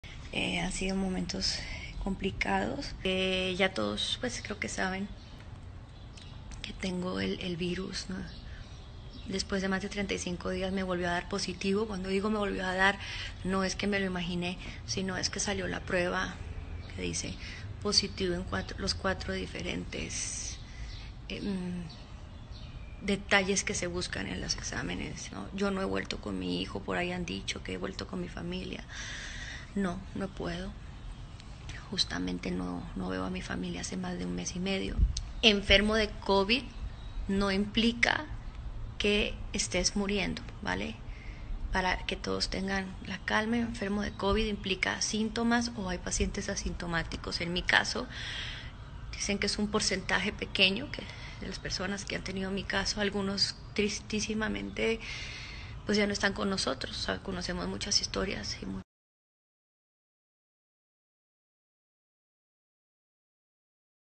El extraño caso de covid-19 de Danna García: la actriz asegura que los médicos no le dan respuestas
Luego de informar el pasado viernes que había dado nuevamente positivo a la prueba, este domingo la actriz respondió preguntas de sus fans a través de Instagram. Asegura que no encuentra respuestas a su caso, que dice, “no es normal” y será estudiado por especialistas.